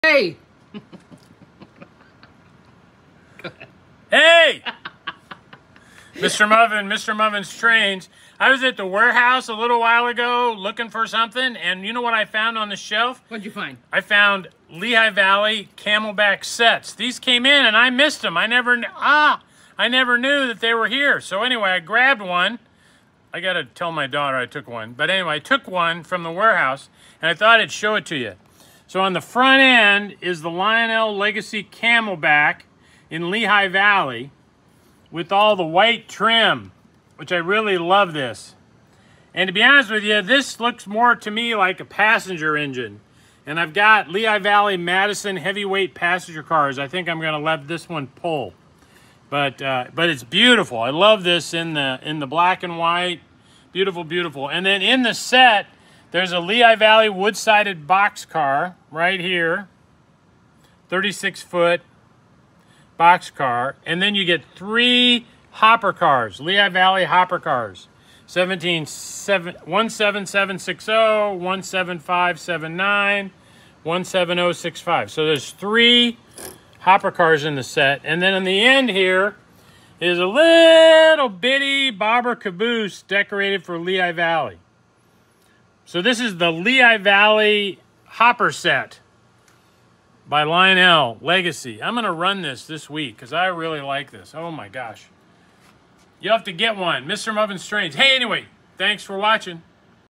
Lionel ships their Lehigh Valley Freight Set with a Camelback steam engine. MrMuffin checks it out!
Lionel offered a bunch of Lagacy Camelback Switchers this fall, and we have had a lot of them. Plus, we had them make us 40 engines unlettered so we could have them lettered and weathered in our customers favorite road names - done by world famous master model railroad artist Harry Hieke. Anyway, here we are showing the Lehigh Valley camelback in the Lionel LV Freight Set - really terrific.